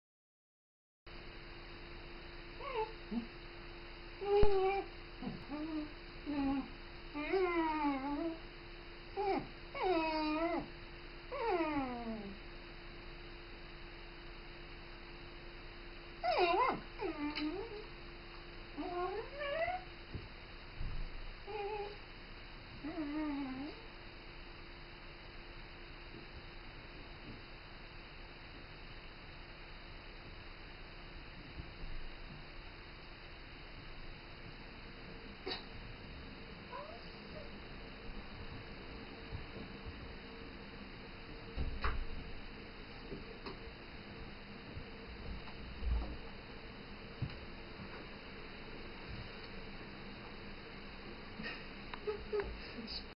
ダンディディンモントテリアのごん太🐶甘鳴き
母を呼んで甘鳴きしてるごん太です